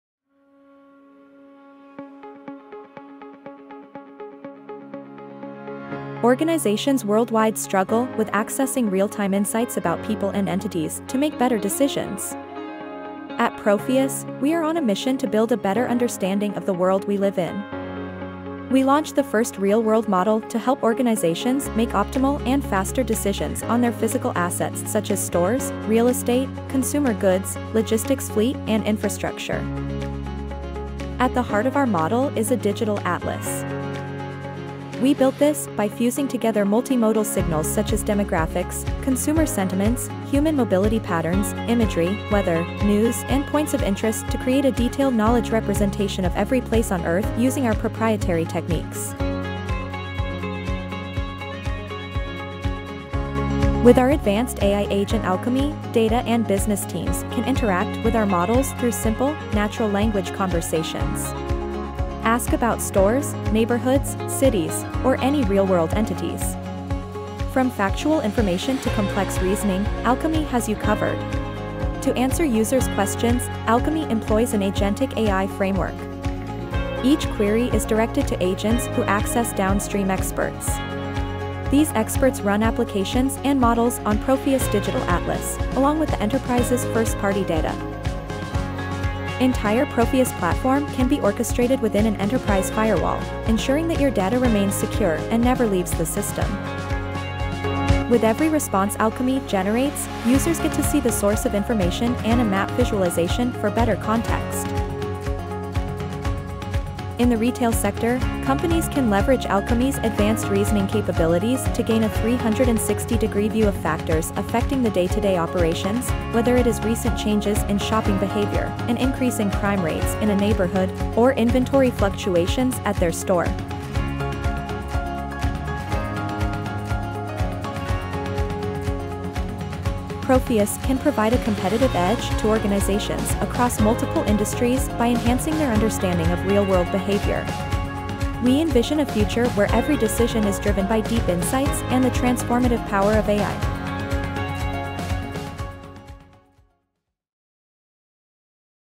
Propheus - Understand the Planet
Propheus: Real World Models for Enterprises.

Our Digital Atlas fuses multimodal data to provide better insights into physical assets like stores, consumer goods, real estate & logistics fleet. Integrate our AI agent, Alchemy, with your LLM to query, reason, and predict with ease.